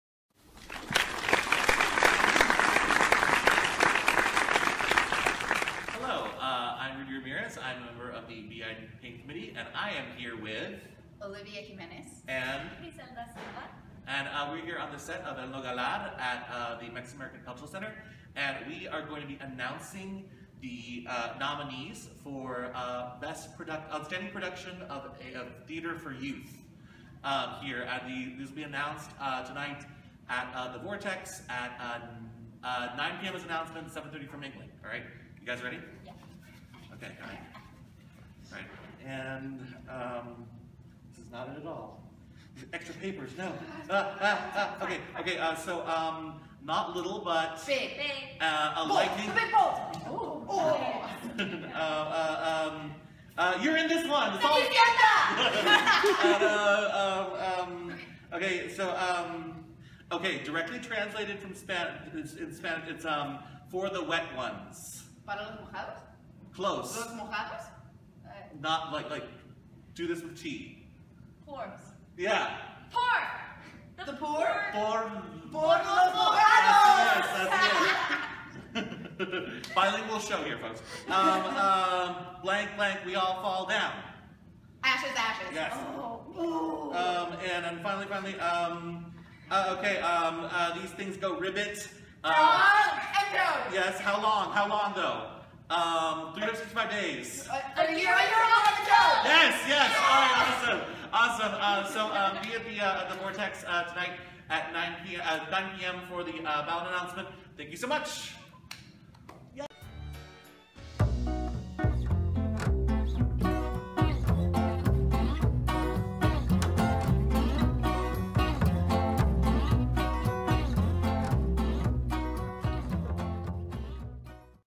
2014/2015 B. Iden Payne Award Nominees for Outstanding Production of Theatre for Youth
We received a little help from the cast of El Nogalar: Olivia Jiménez and Gricelda Silva, with Rudy Ramirez.